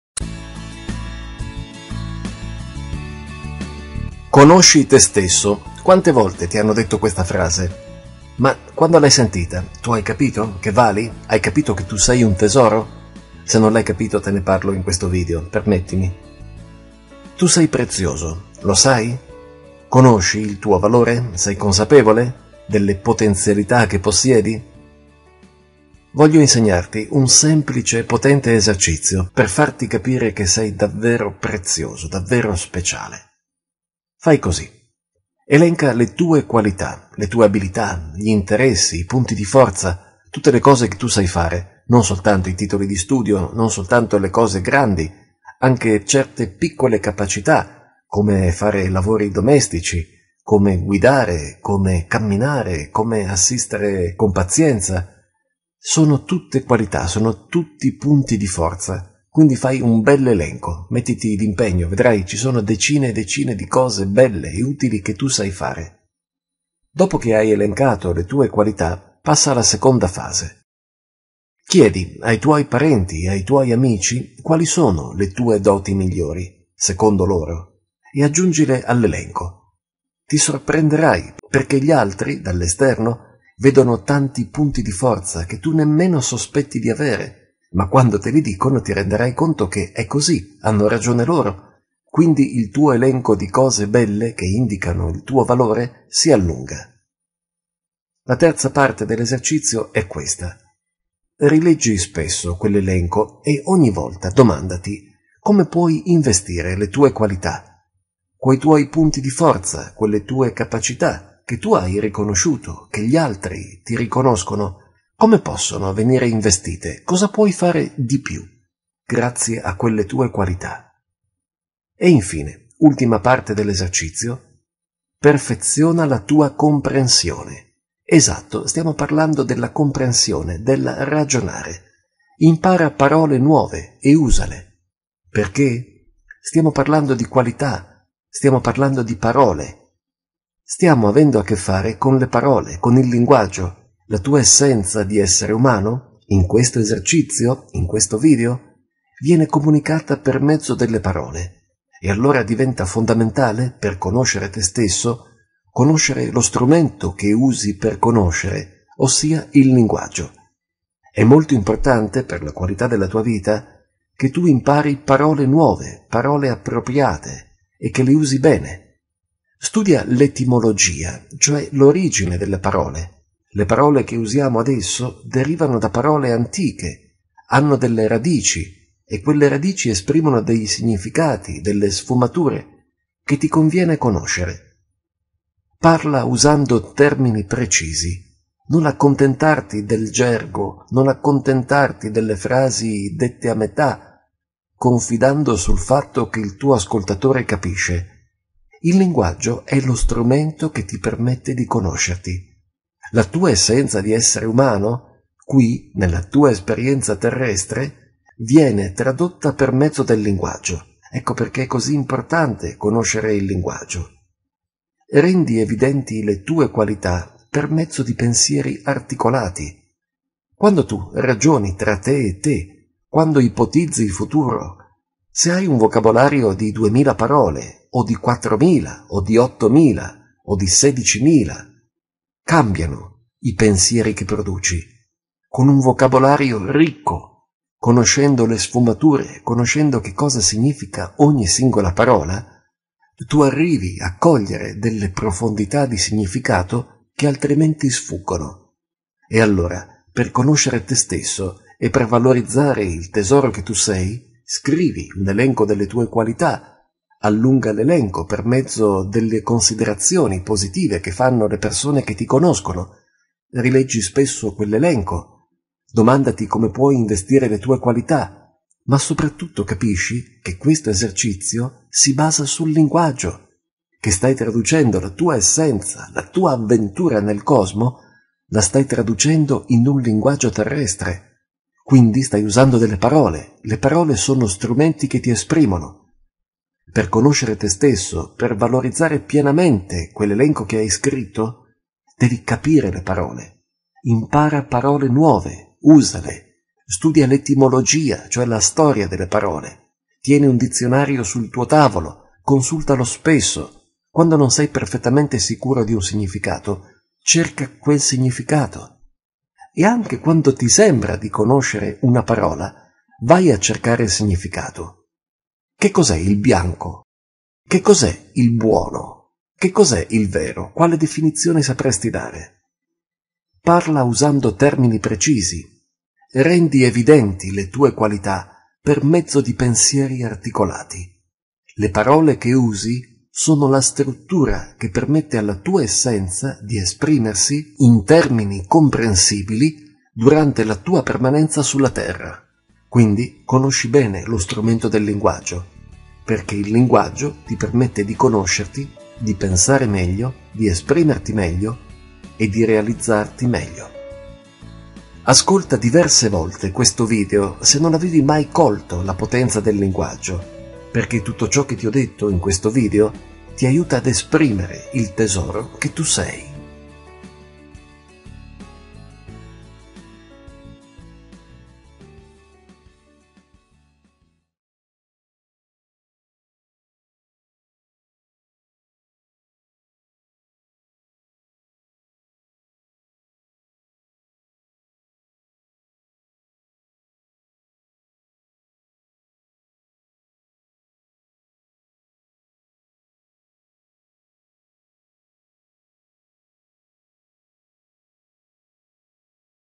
Conosci te stesso, il tesoro che tu sei
CLVQTS - Come La Vita Quando Ti Sorride

Quante volte hai sentito
la frase "conosci te stesso"?

Che cosa ti ha ispirato, quella frase?

Tra i molti significati che puoi
ricavare da essa, vi è anche
la possibilità di accorgersi
che ciascuno di noi rappresenta
un potenziale unico,
al suo specifico livello.

In questo video ti porto a considerare
alcuni aspetti della tua essenza
da valorizzare per mezzo di uno strumento
speciale, che non conosci a fondo.

Per saperne di più visita il sito
Life Coaching Seminari Corsi Online Consulti
Soluzioni Per Vivere Meglio